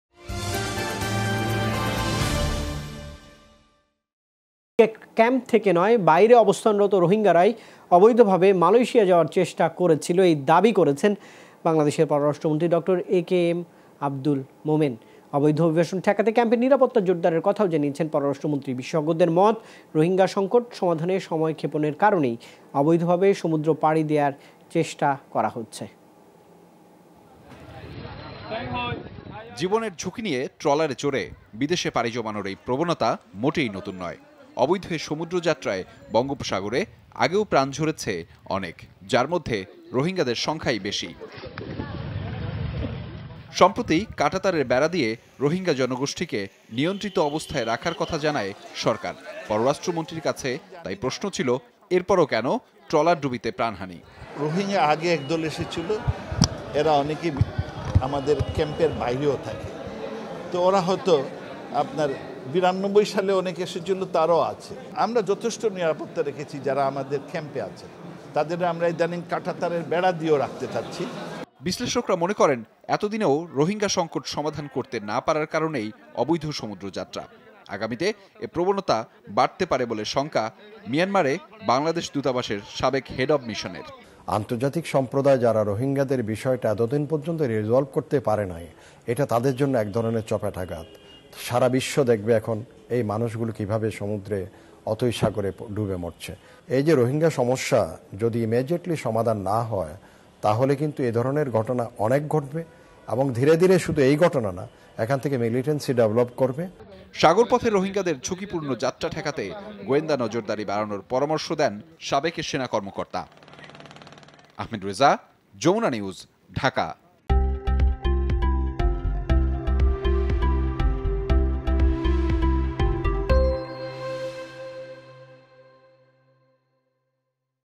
ক্যাম্পের বাইরে অবস্থানরত রোহিঙ্গারাই অবৈধভাবে মালয়েশিয়া যাচ্ছিলো-পররাষ্ট্রমন্ত্রী | Jamuna TV
ক্যাম্প থেকে নয়, বাইরে অবস্থানরত রোহিঙ্গারাই অবৈধভাবে মালয়েশিয়া যাওয়ার চেষ্টা করছিলো। এ দাবি করেছেন পররাষ্ট্রমন্ত্রী ড. এ কে আব্দুল মোমেন। অবৈধ অভিবাসন ঠেকাতে ক্যাম্পের নিরাপত্তা জোরদারের কথাও বলেন তিনি। বিশেষজ্ঞদের মত, রোহিঙ্গা সংকট সমাধানে সময়ক্ষেপণের কারণেই অবৈধভাবে সমুদ্র পাড়ি দেয়ার চেষ্টা করা হচ্ছে।